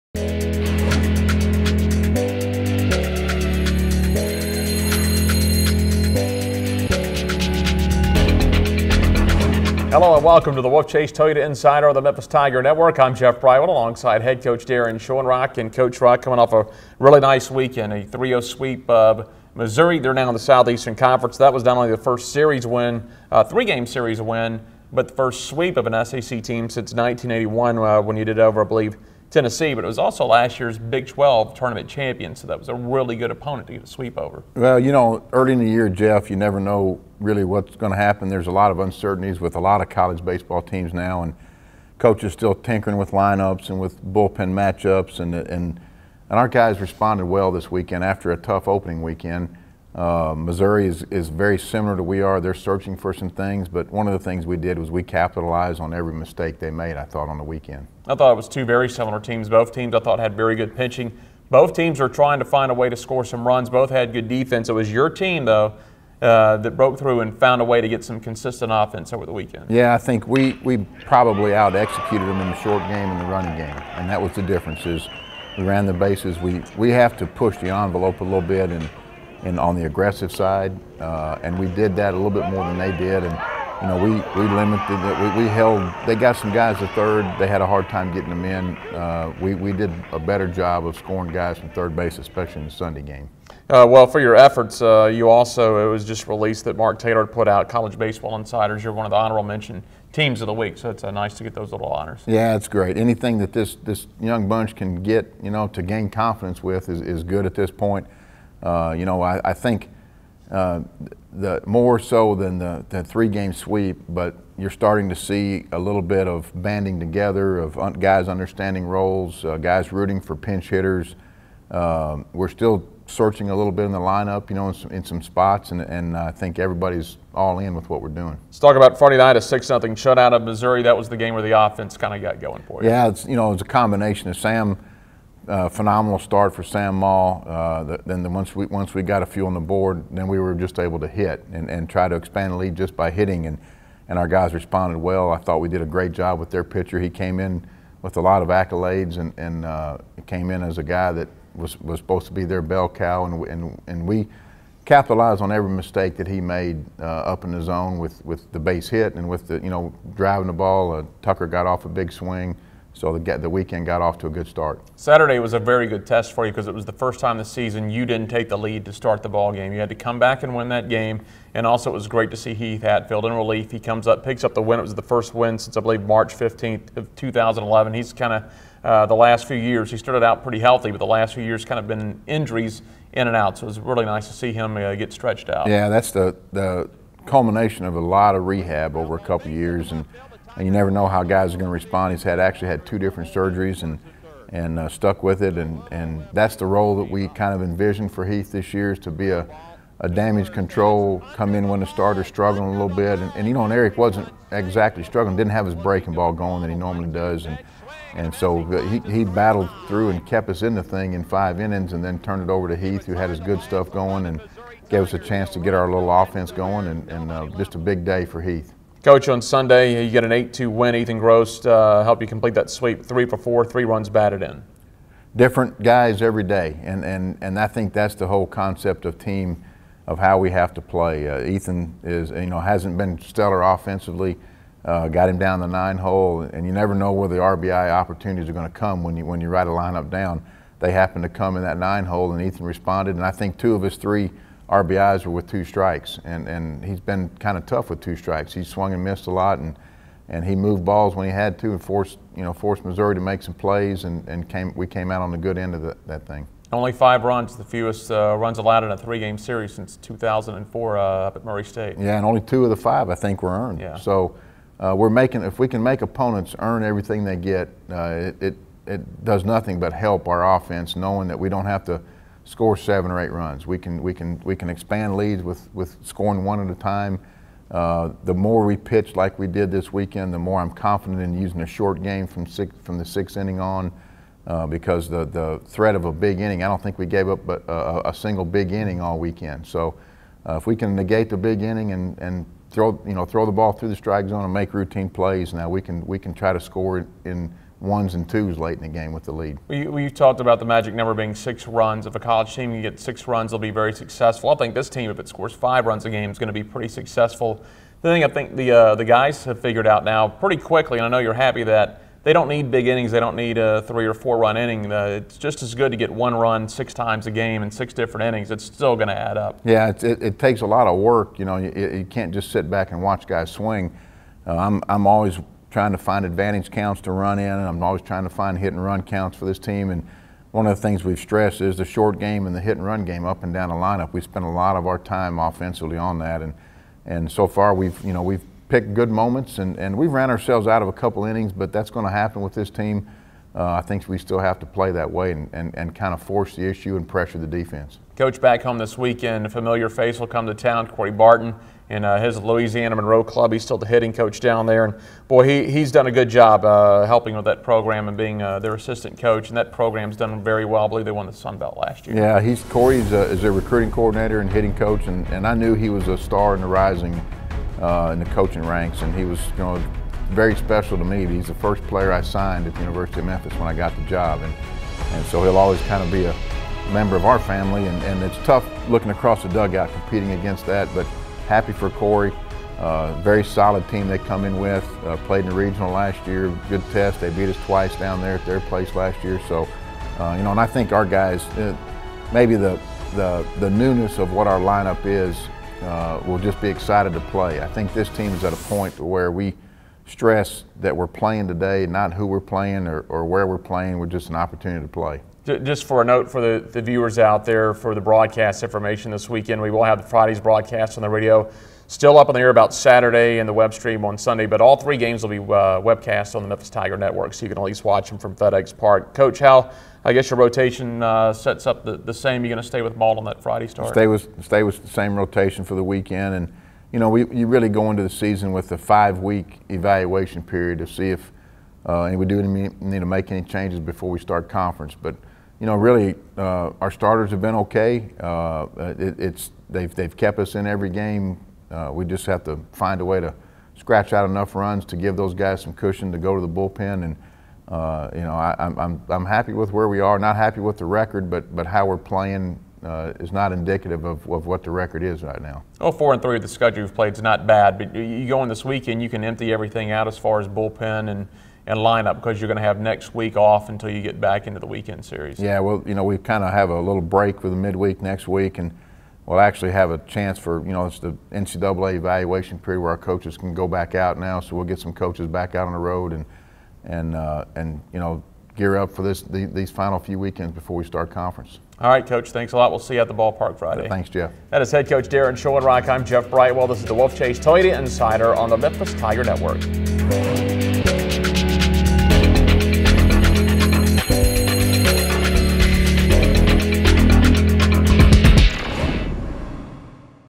Wolfchase Toyota Insider: Memphis Baseball
Episode 62: Jeff Brightwell talks with Memphis Baseball Head Coach Daron Schoenrock about the series against SEC foe Missouri and previews the upcoming series with Louisiana-Monroe March 1st-3rd.